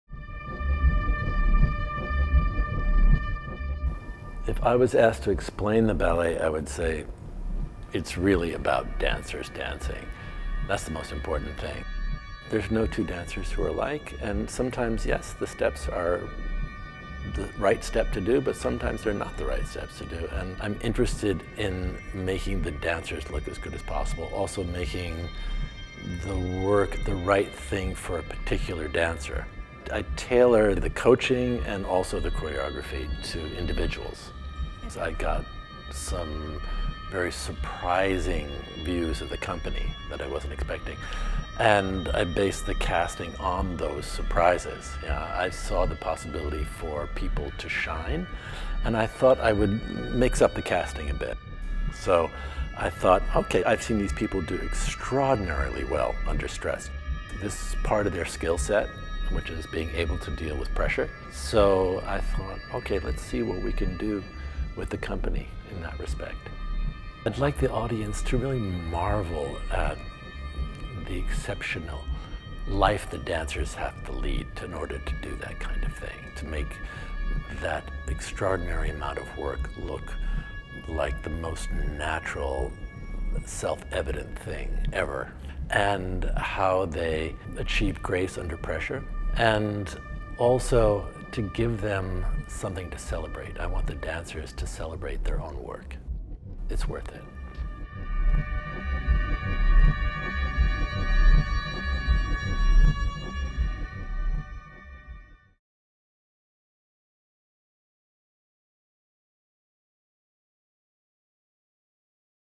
INSIDE LOOK | William Forsythe's Pas/Parts 2018
Choreographer William Forsythe shares his perspective on bringing Pas/Parts 2018 to the Boston Opera House, Mar 9–Apr 7